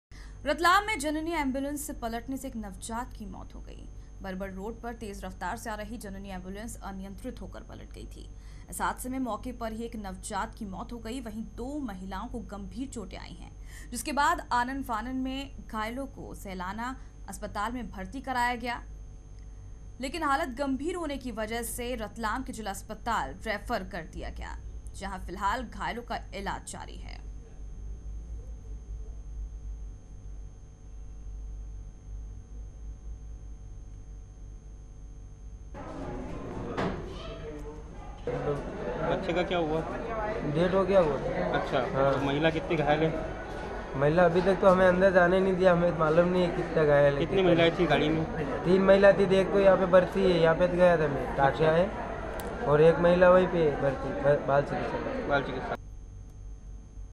जननी एंबुलेंस पलटने से नवजात की मौत,दो घायल - MP Patrika
रतलाम में जननी एंबुलेंस पलटने से एक नवजात की मौत हो गई. बरबड़ रोड़ पर तेज रफ्तार से आ रही जननी एंबुलेंस अनियंत्रित होकर पलट गई. इस हादसें में मौके पर ही 1 नवजात की मौत हो गई. वहीं दो महिलाओं को गंभीर चोटे आई है .जिसके बाद आनन-फानन में घायलों को सैलाना अस्पताल में भर्ती कराया गया...लेकिन हालत गंभीर होने की वजह से रतलाम के जिला अस्पताल रेफर कर दिया गया. जहां फिलहाल घायलों का इलाज जारी है.